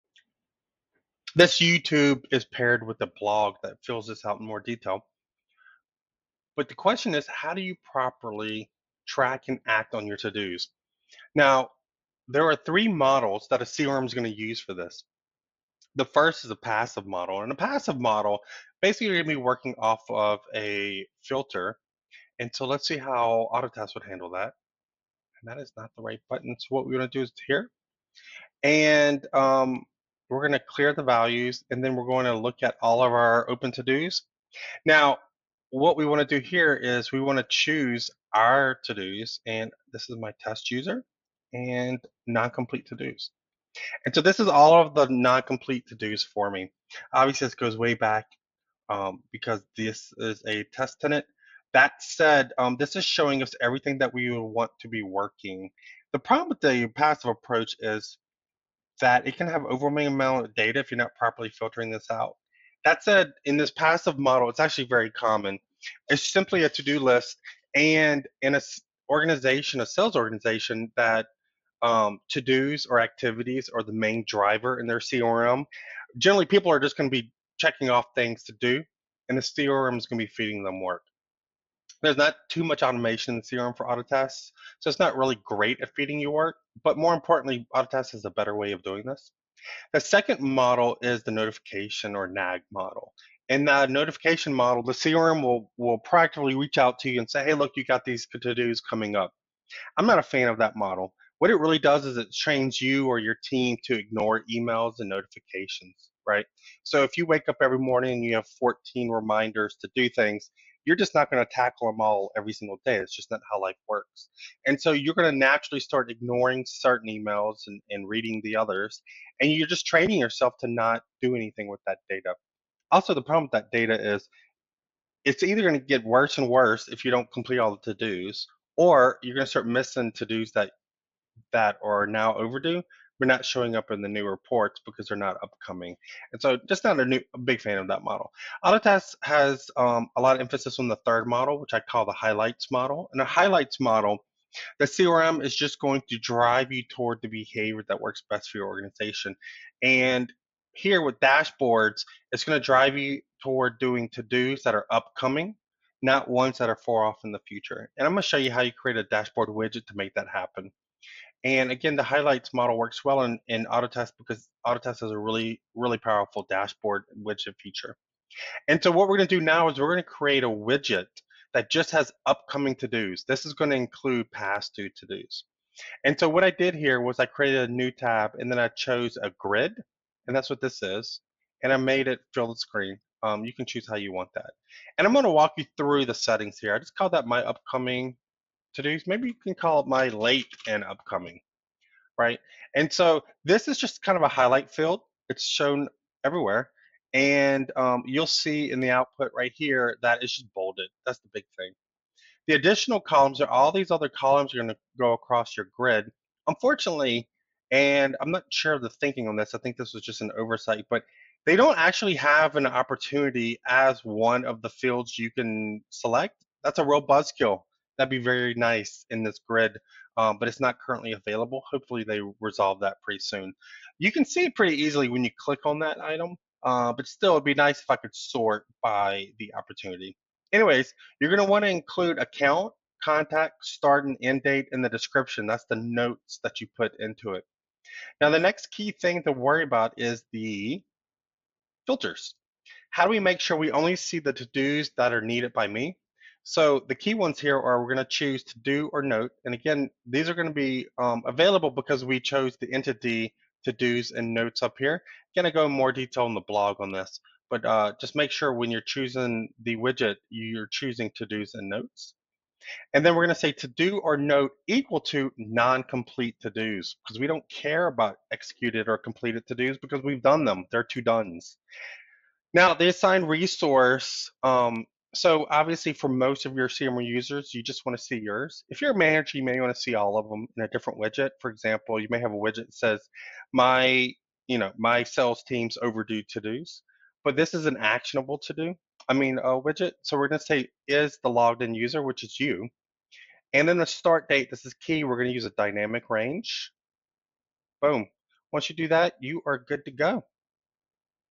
How to be notified when an Autotask CRM ToDo is Due
In this video, I'll walk you through the process of tracking and completing Autotask CRM ToDos. You can also read the blog for full details